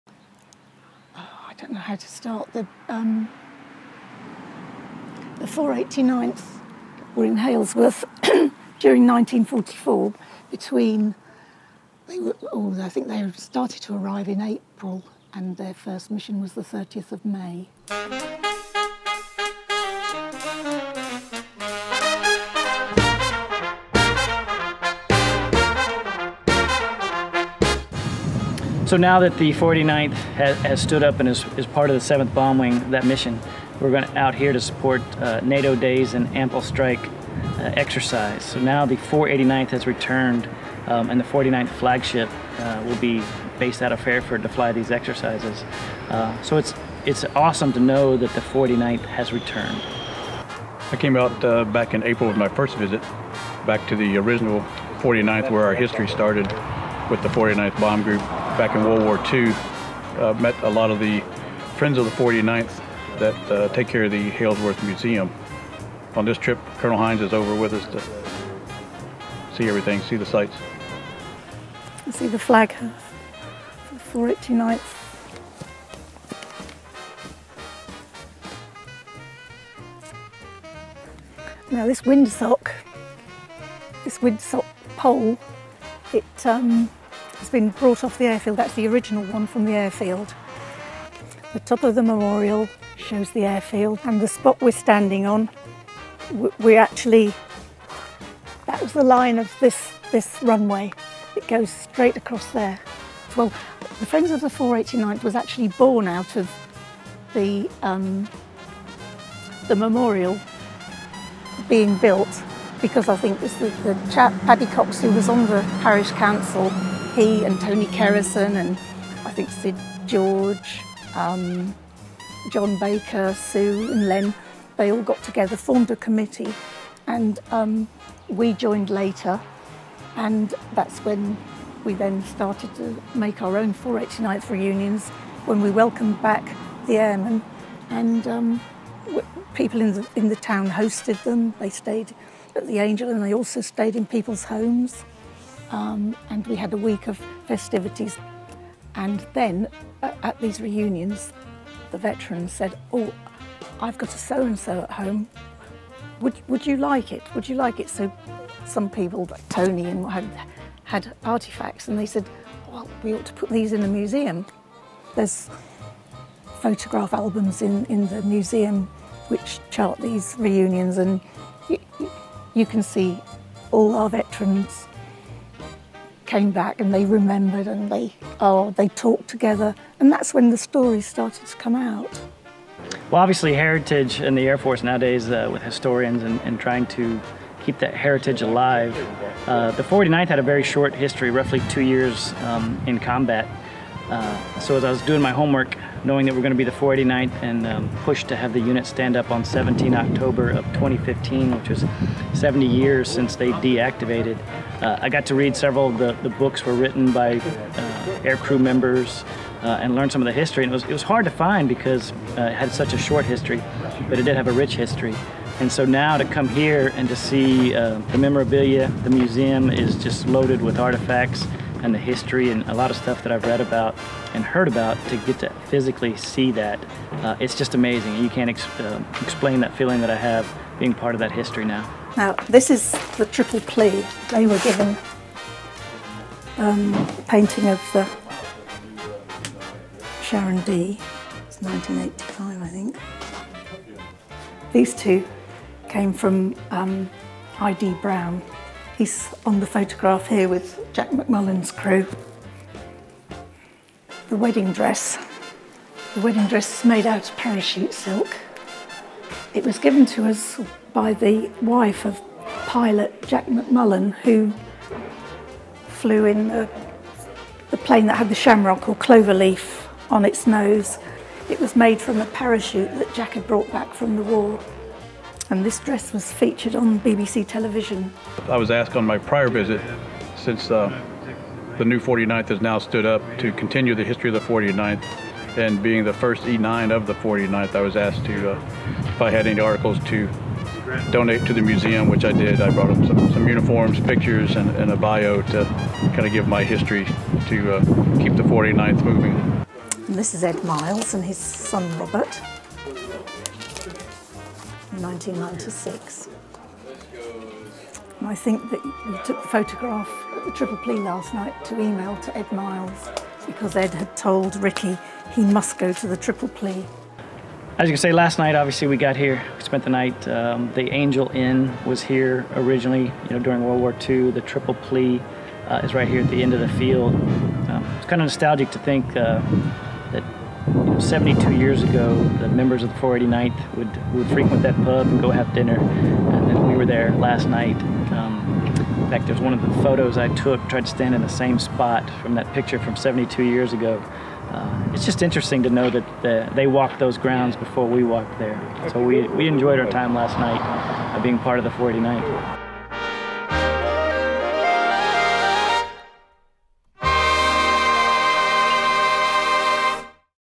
Return of the 489th
After 72 years away, the 489th Bomb Group has returned to England!
While the 489th BG was in England supporting exercise AMPLE STRIKE, it had the opportunity to connect with its WWII heritage. Watch this video to learn all about it!